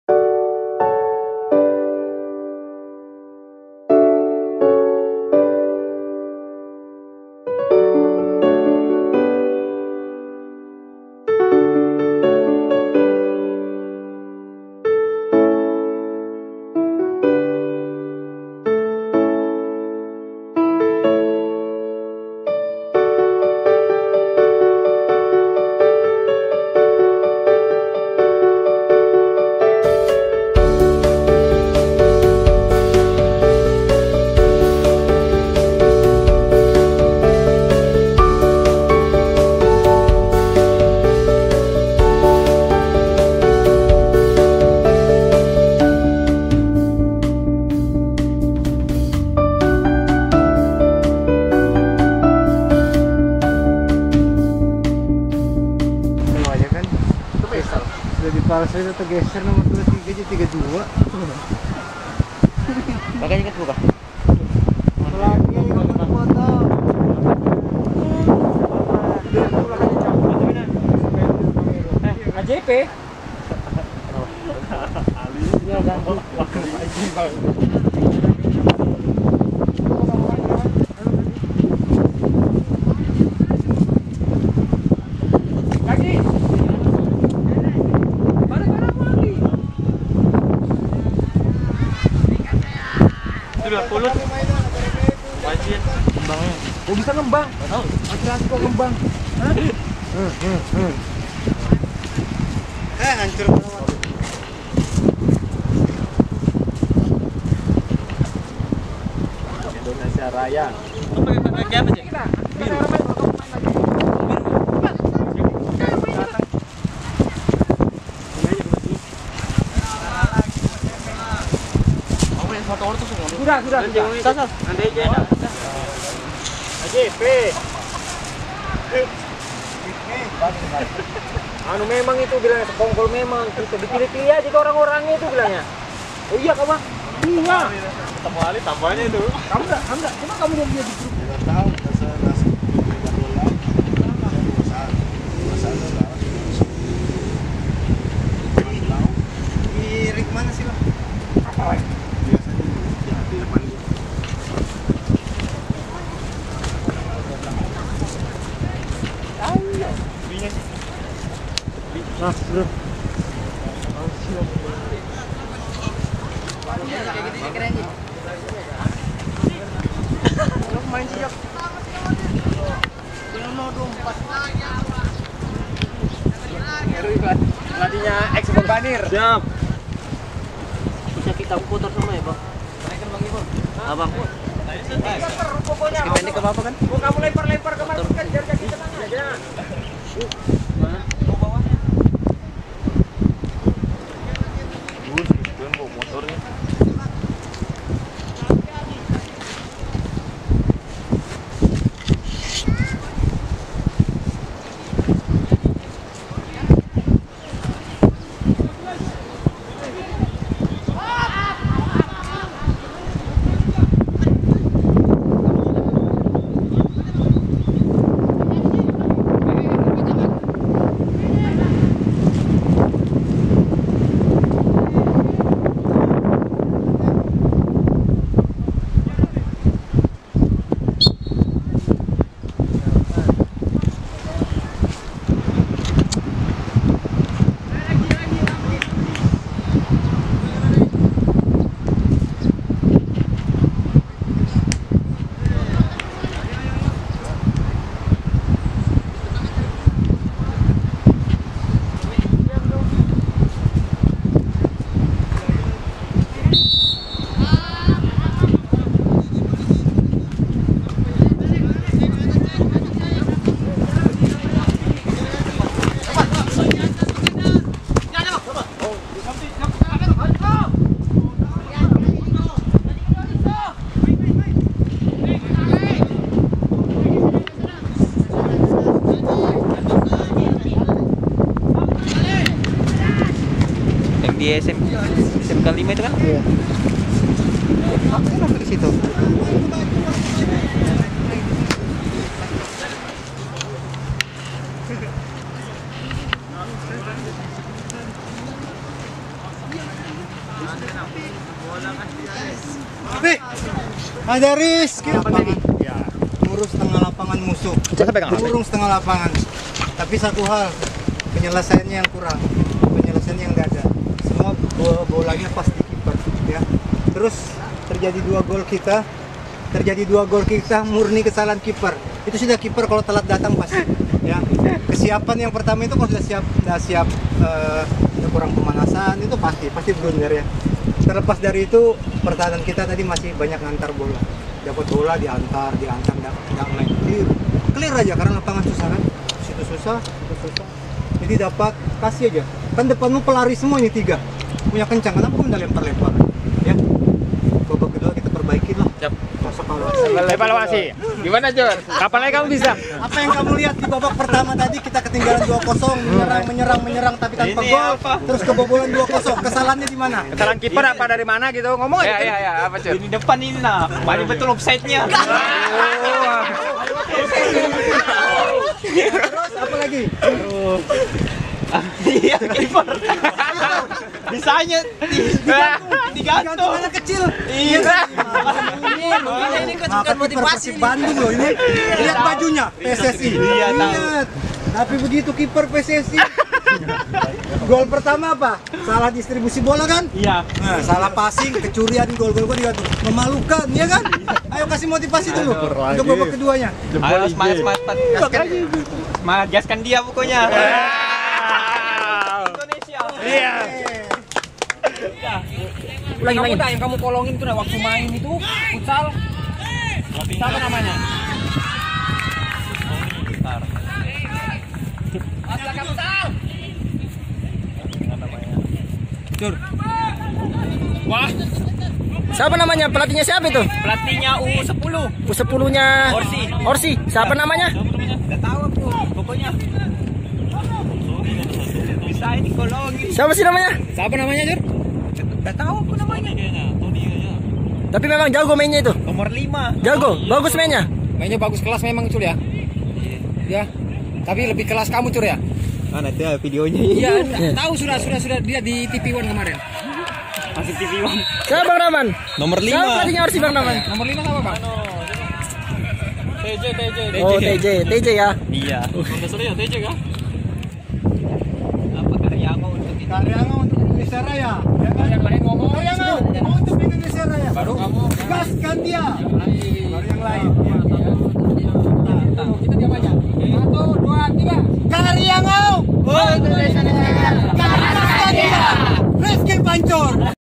Idola Balikpapan Hadir Dalam Laga Penutup Silaturahmi Cup | Matchday Diary (Chapter 8)
Idola Balikpapan, Riski Pancur hadir dalam laga antara Kariangau vs Guntur Putra, Kedepannya Riski Pancur Akan meluangkan waktu untuk bermain bersama dengan Kariangau.

JANGAN LUPA!
Dukung terus channel kami, 🙏
Agar kita lebih semangat membuat konten-konten Kariangau selanjutnya!